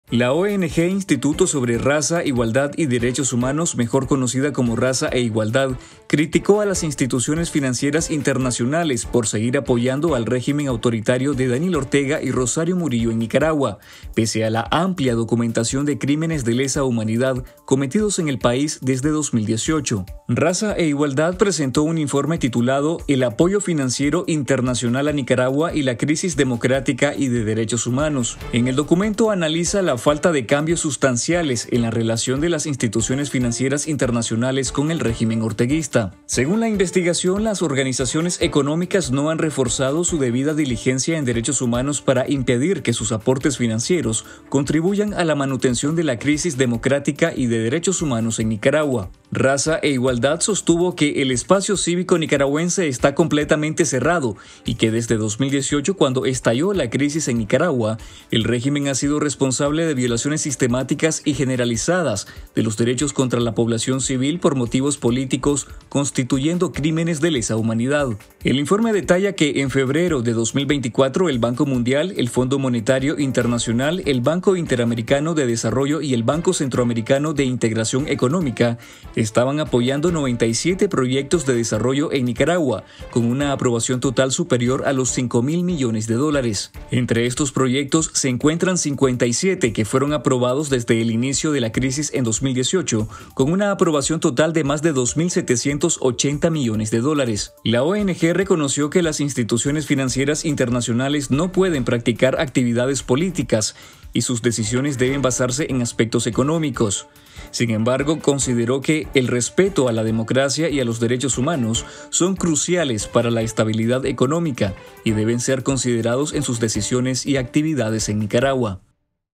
Critican a instituciones financieras por apoyar a la dictadura
La oenegé Instituto sobre Raza, Igualdad y Derechos Humanos, mejor conocida como Raza e Igualdad, criticó a las instituciones financieras internacionales por seguir apoyando al “régimen autoritario” de Daniel Ortega y Rosario Murillo en Nicaragua, “pese a la amplia documentación de crímenes de lesa humanidad cometidos en el país desde 2018”.

Sigue la cobertura de la Asamblea General de la OEA, desde Paraguay, en nuestro web confidencial.digital 👉🏻